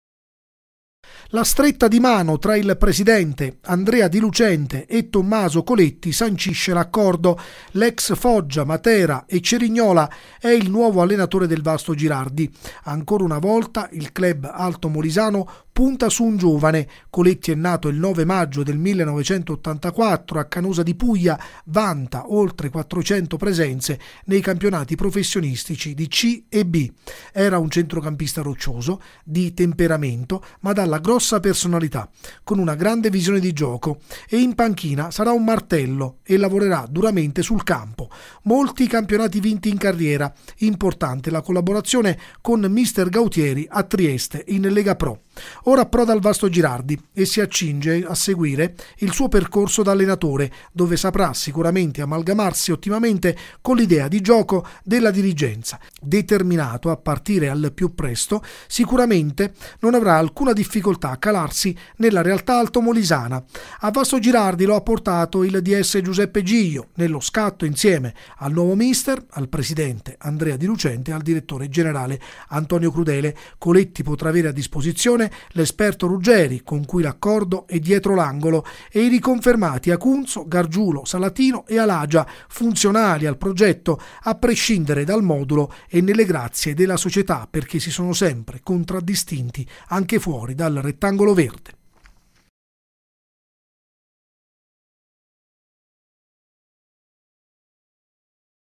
Tommaso Coletti nuovo allenatore del Vastogirardi - 09/07/2022
Servizi TG | Tommaso Coletti nuovo allenatore del Vastogirardi | 9/7/2022